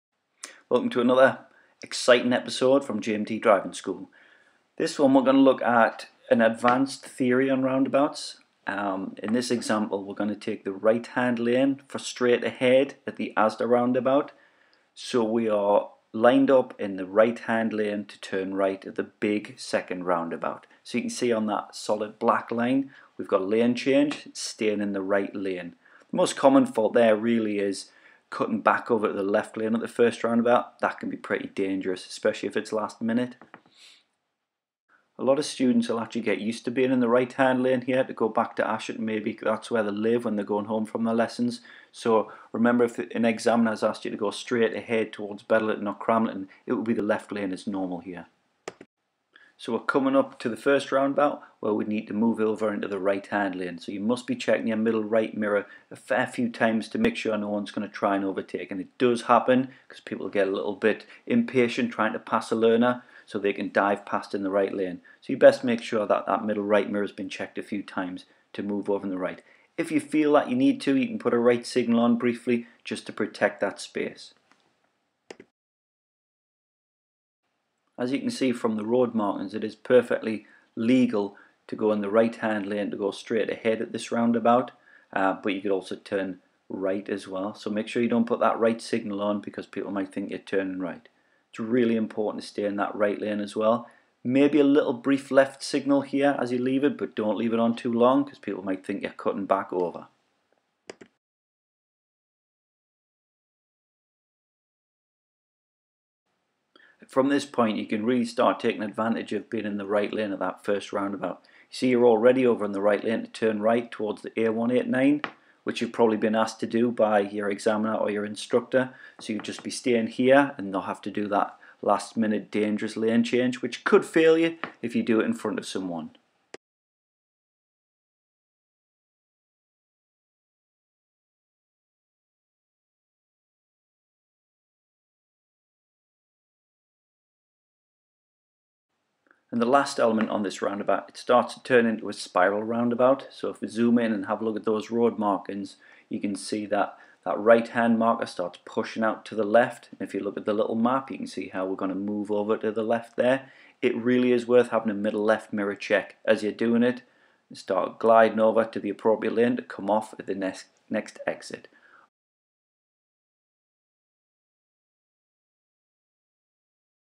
Advanced roundabout theory (double roundabout)
Some tips to help you deal safely with roundabouts that are close together.